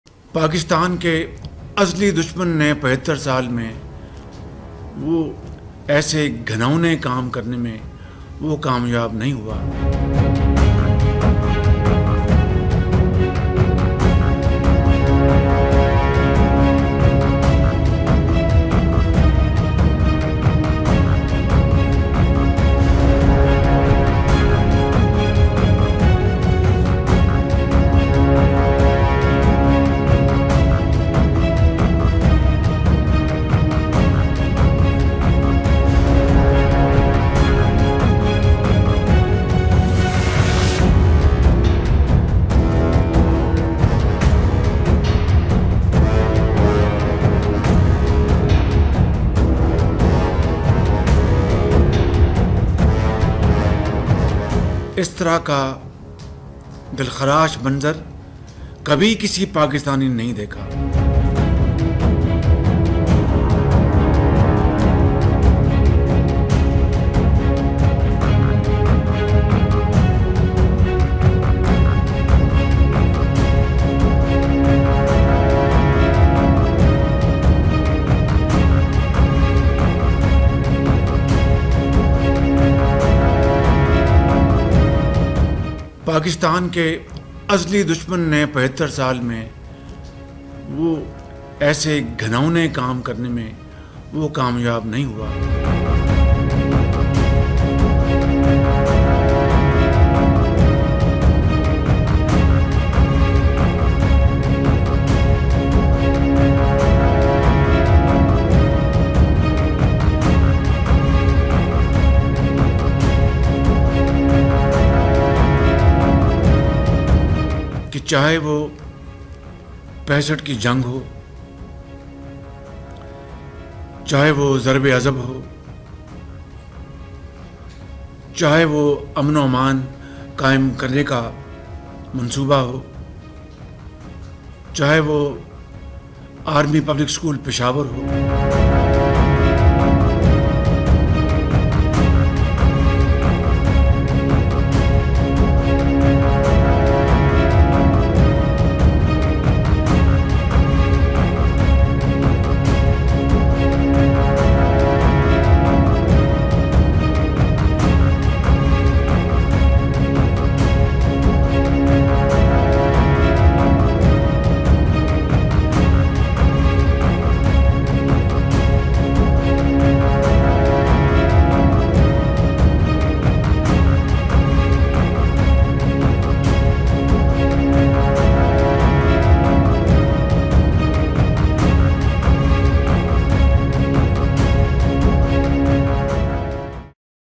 Shehbaz Sharif Indirectly Refers To India As ‘Real Enemy’ In Speech Slamming Pak's Free Imran Riots
Pakistan PM Shehbaz Sharif has indirectly dragged in India into the Imran Khan mess. Why did Sharif cry about 'real enemy'? Watch the video to find out more.